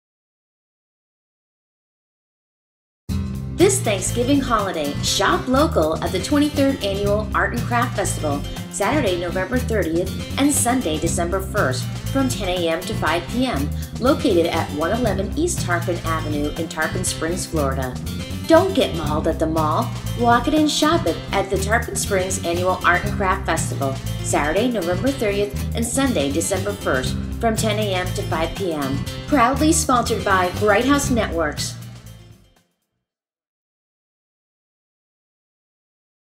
Tarpon Springs Art and Craft Festival
This Thanksgiving Holiday
'Shop Local' at the 23 Annual Art and Craft Festival
Saturday, November 30 & Sunday, December 1
from 10 a.m. -- 5 p.m.
Located at 111 East Tarpon Avenue in Tarpon Springs Florida

DON'T GET MAULED AT THE MALL! Walk it and shop it at the
Tarpon Springs annual Art and Craft Festival
Tarpon Springs outdoor showcase features one of a kind gift items
for your holiday shopping or... go ahead and buy something
for yourself. You wont resist!

It's the perfect opportunity for locals and visitors alike to shop
for their entire gift giving needs.
Saturday November 30th and Sunday, December 10th